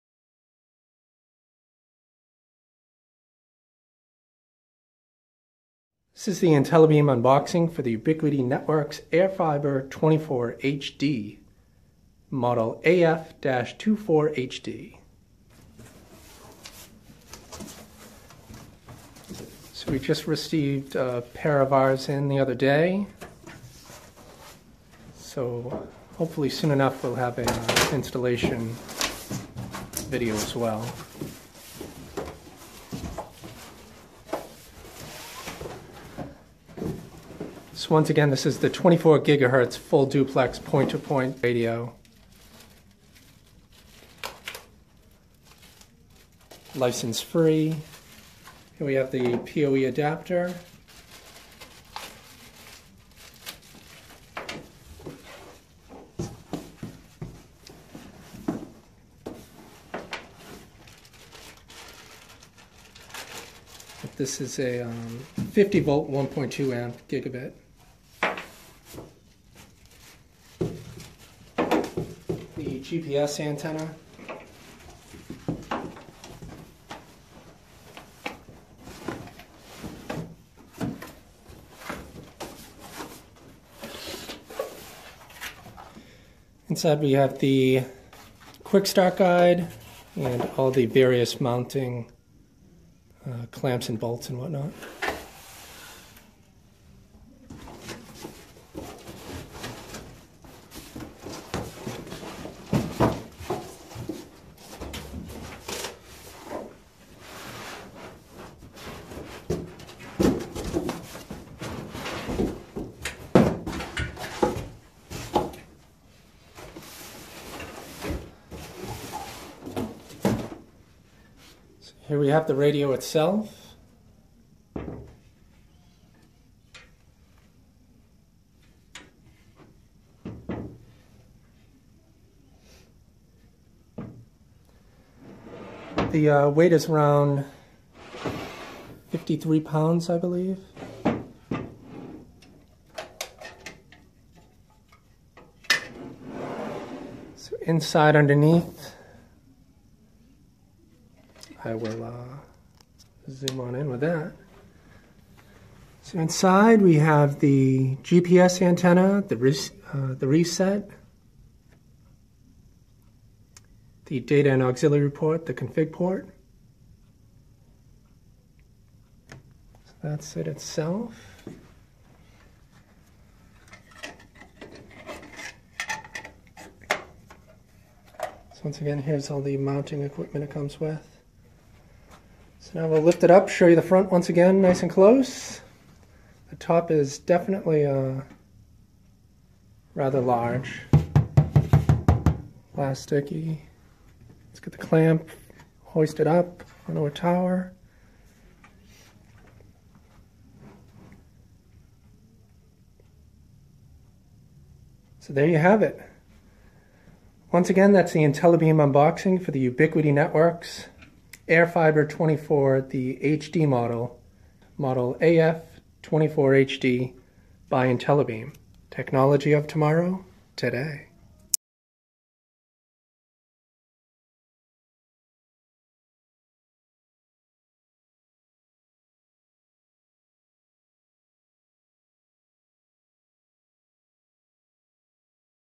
Ubiquiti Networks airFiber 24 HD (AF24HD) unboxing by Intellibeam.com
24 GHz Full Duplex Point-to-Point 2 Gbps Radio
Model: AF24HD

Featuring INVICTUS™custom silicon for 2+ million packets-per-second processing, the airFiber®24HD is ideal for outdoor, PtP bridging and backhaul applications requiring maximum performance.

Frequency: 24 GHz
Throughput: 2Gbps
Range: 20+ km

by Intellibeam.com
Technology of tomorrow, today™

Facebook.com/intellibeam
Twitter.com/intellibeam